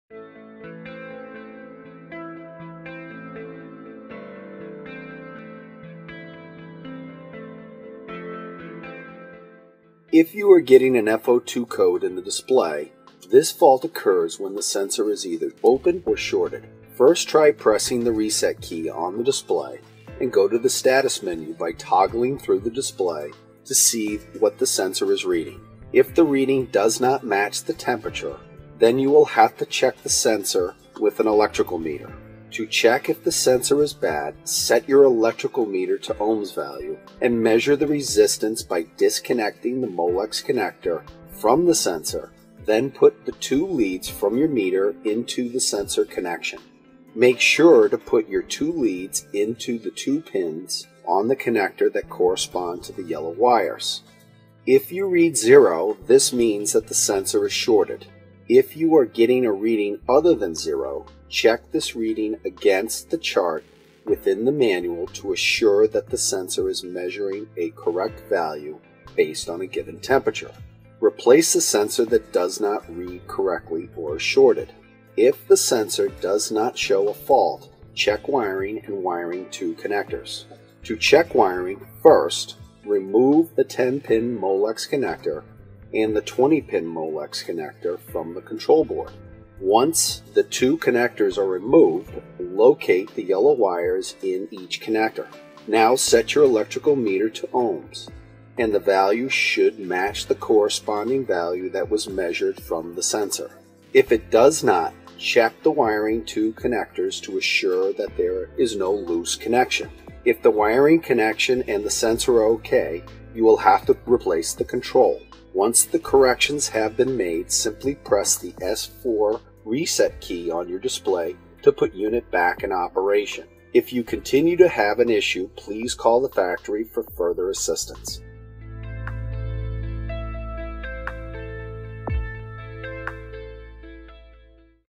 F02 Fault Code for Boilers -
HTP: F02 Fault Code for Boilers: SUPPLY SENSOR: Indicates that the supply temperature sensor of the boiler has failed. This is a serious safety issue and the boiler will not restart until the sensor is replaced by a technician and he pushes the RESET button on the display. This situation is indicated by the red light on the display and the flashing word LOCKOUT. During this lockout fault, the pump will be on as indicated on the second line of the display.

1. Check the electrical connection to the thermistor on the outlet manifold. Verify 5 VDC by checking in Molex connector. If there is no 5 VDC, check the harness. If harness is OK, replace control. NOTE: The boiler will reset automatically. Verify thermistor values by referencing chart in this manual.
2. Replace thermistor if necessary.